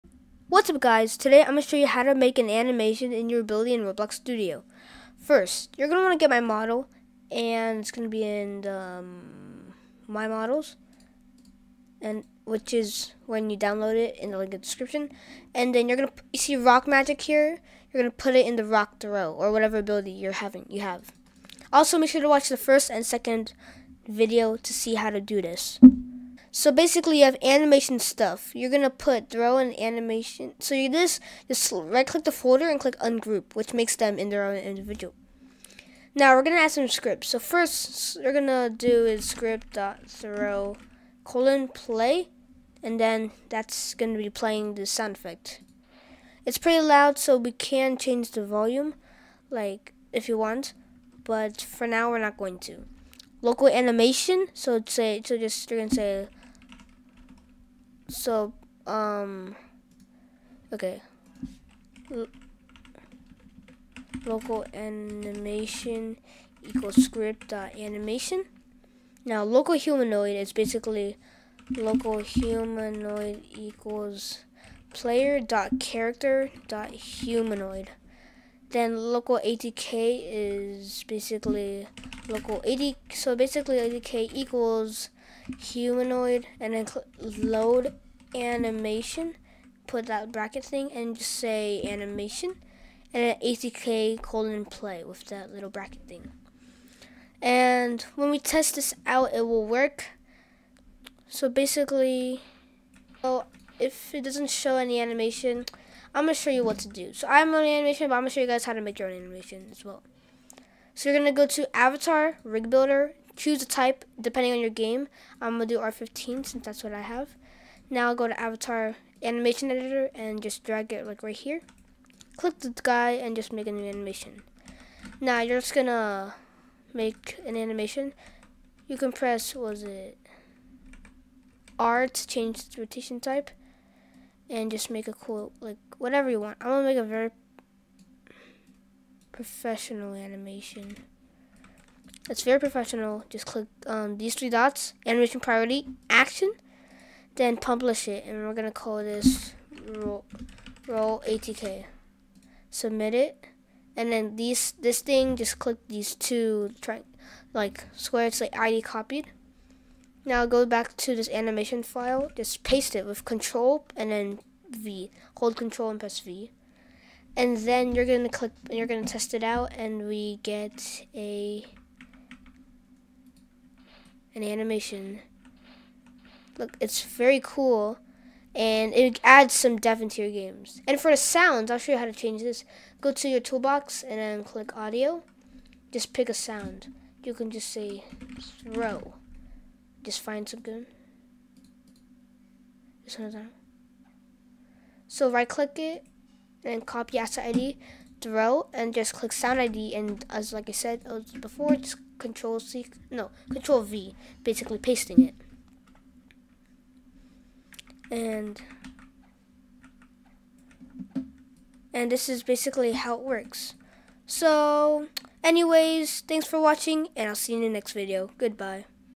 Attack Animation & Sound Tutorial | Ability in Roblox Studio #3
This video shows how to make an animation and sound effect in your roblox studio attacks!